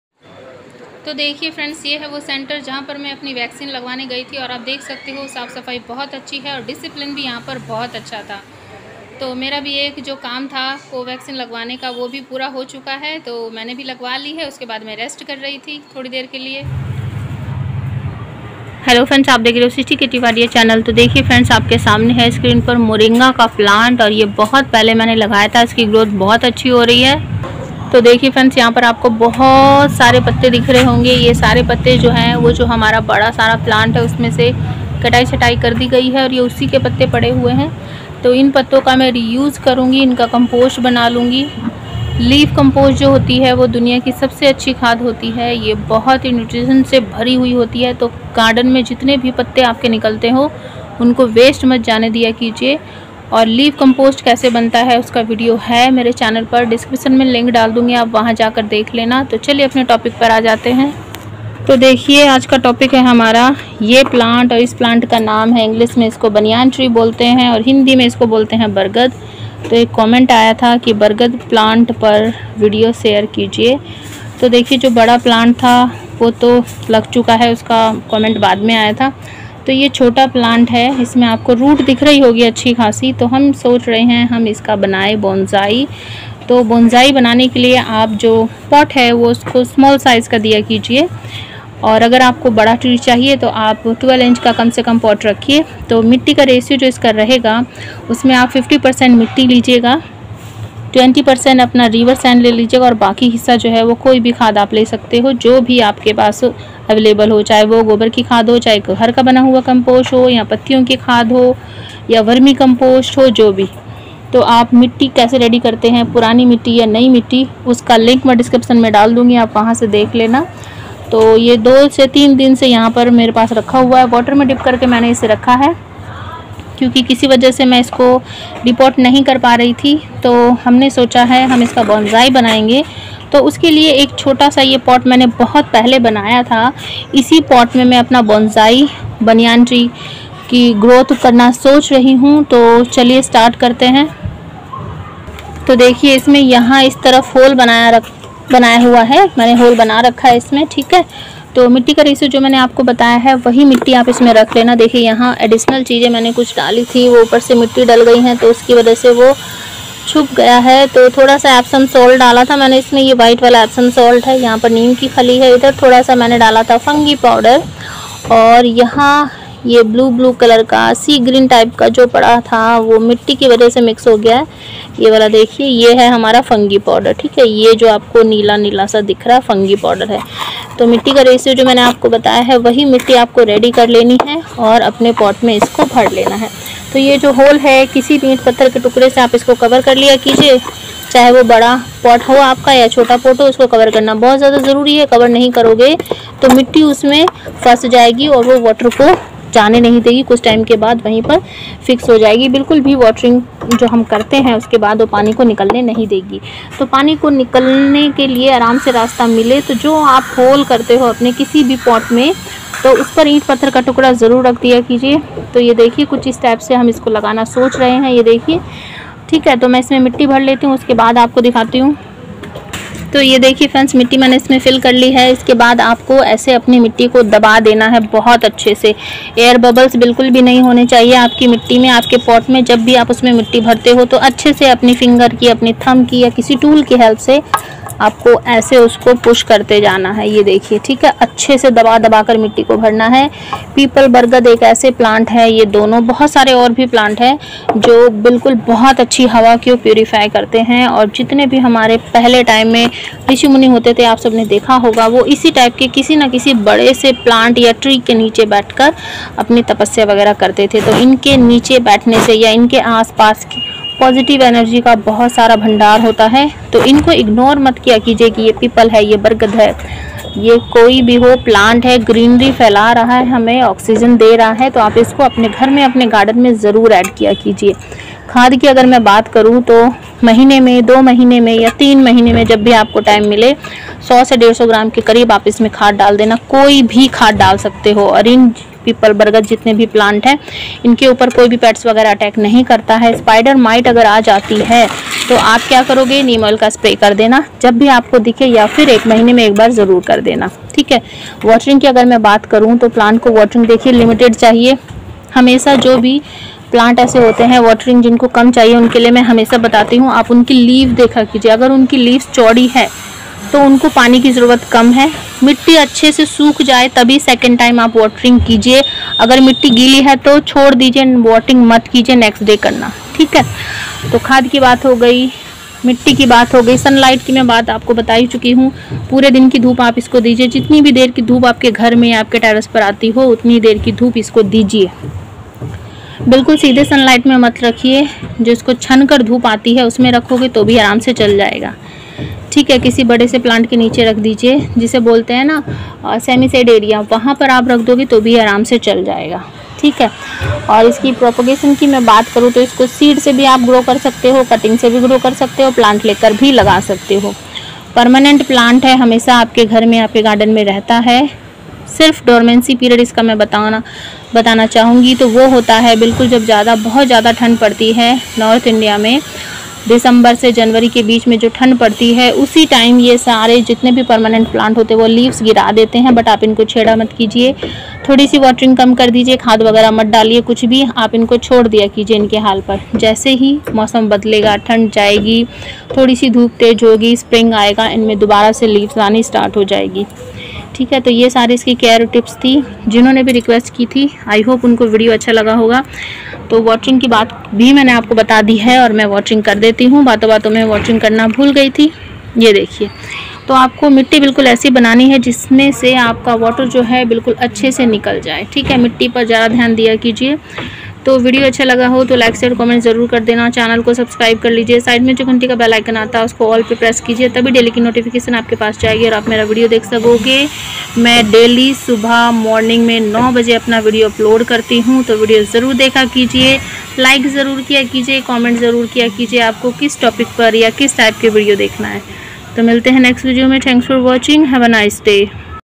||Requested video|| reporting care tips of baniyan tree
Hello friends aj ka video share krne ki request aae thi aj ke video me main apko india ka national tree banyan tree ya plant ki reporting care tips and tricks btaugi friends is plant ko ap apne garden ya ghar me jrur add kre because is plant ke aas paas bahut sari positive energy hoti h ji apko bhi positive bna deti h or ap bhi bahut sare kam krne lg jate ho friends video ko like share comment kre or mere channel ko subscribe kre with bell 🔔 button for daily notification